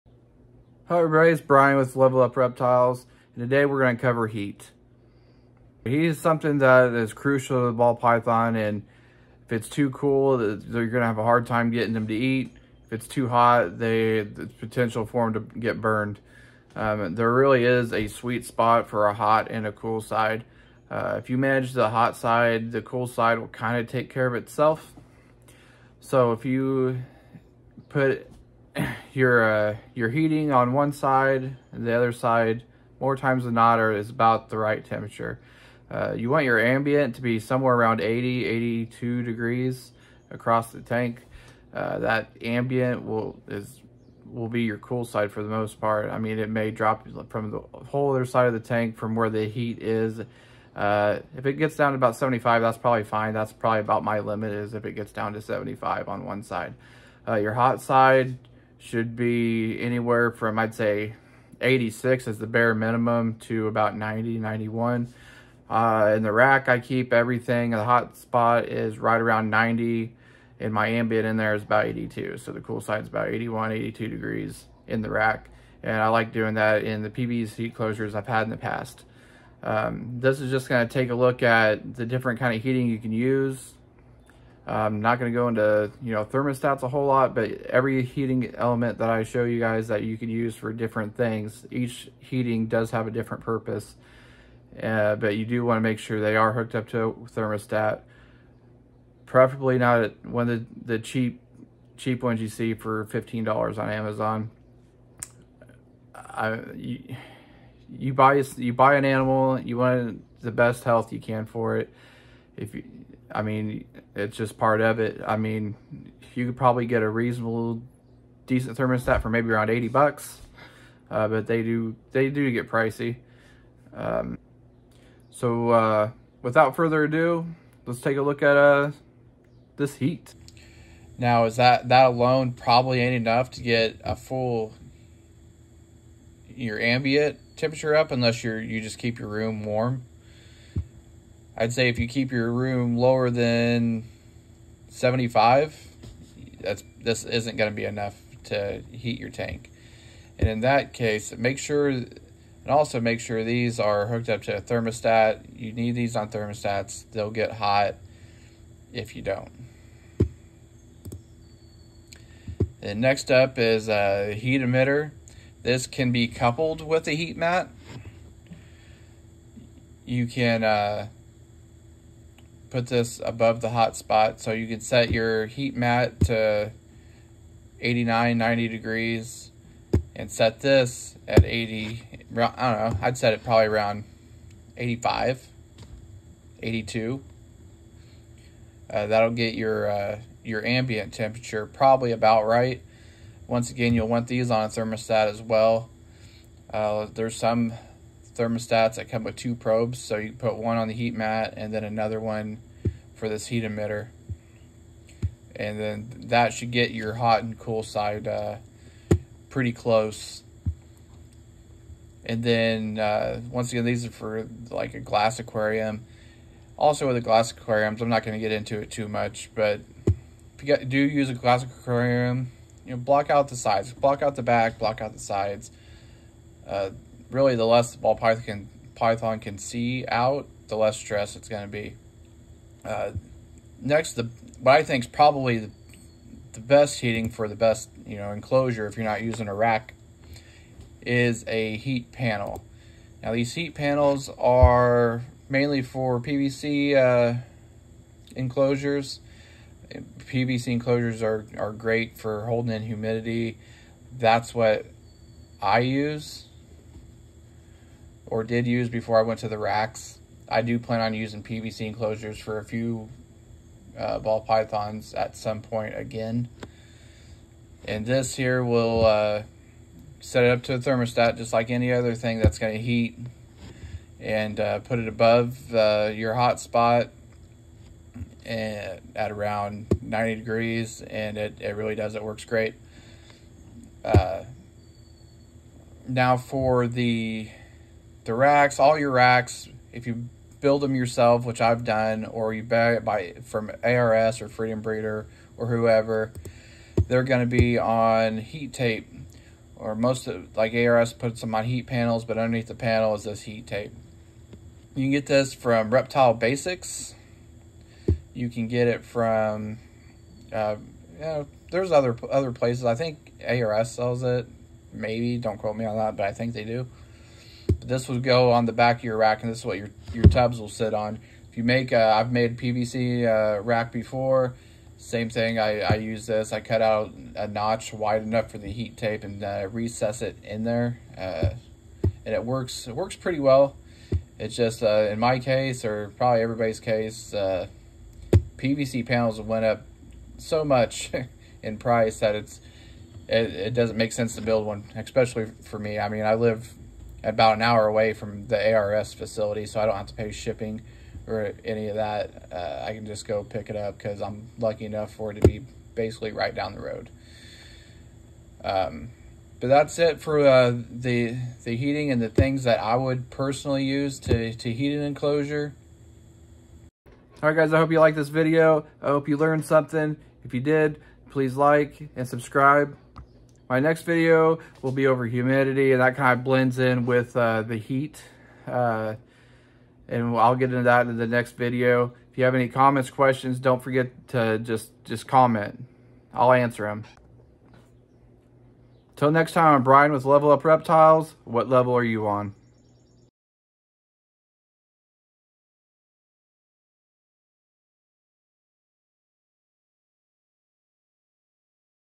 How to Heat a Ball Python Enclosure
In this video I will be going over the different types of heating for your Ball Python!!!